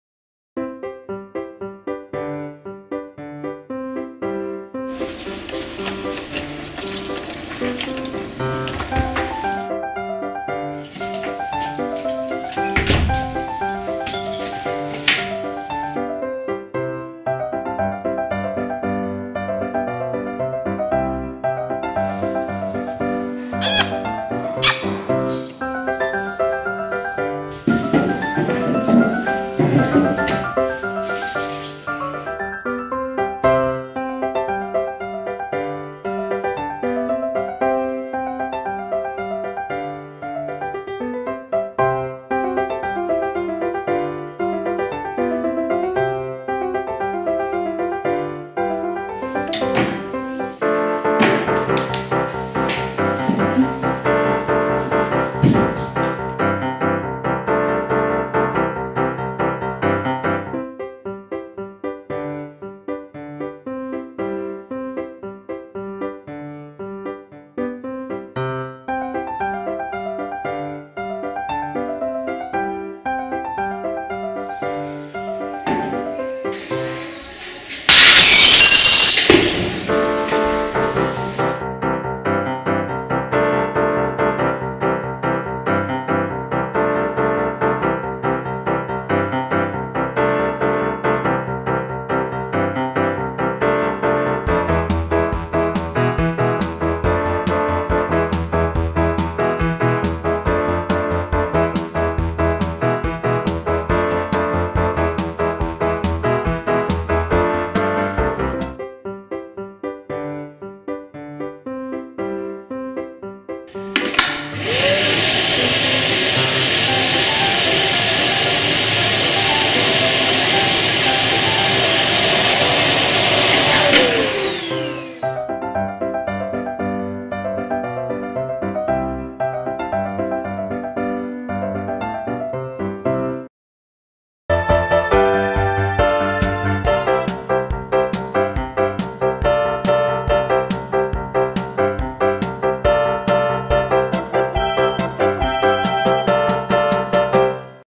Pink Spidergirl and Spiderman Family Life Spiderman after work Real Life Super Heros Life
Spiderman gets home from work, Pink Spidergirl waiting at home. Super Heros Life